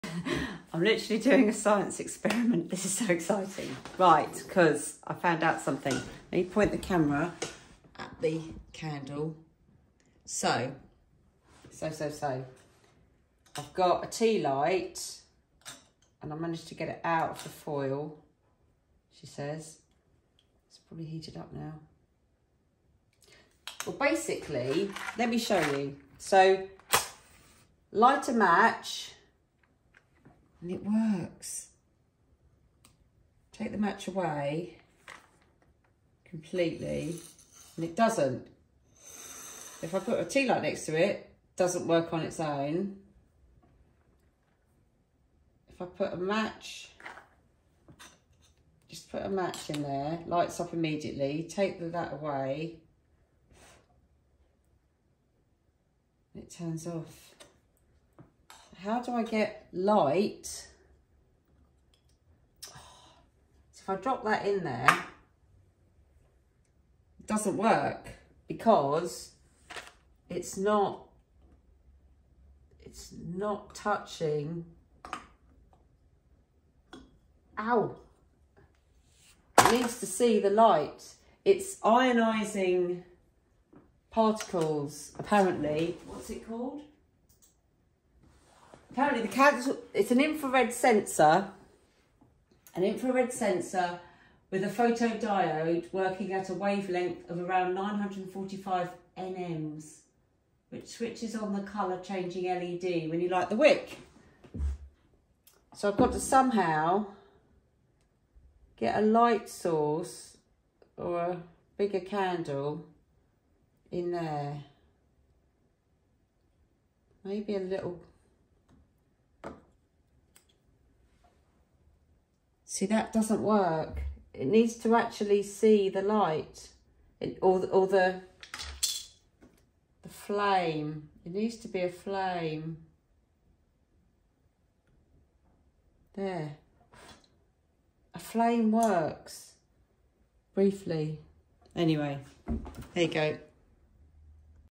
How to make the Marks and Spencer candle light up again
I love my Marks and Spencer light up candle but wanted it to work after the wax had run out.

I’ve managed to work out how I can trigger it but not sure how to do it longer term!

Please comment if you have a foolproof solution!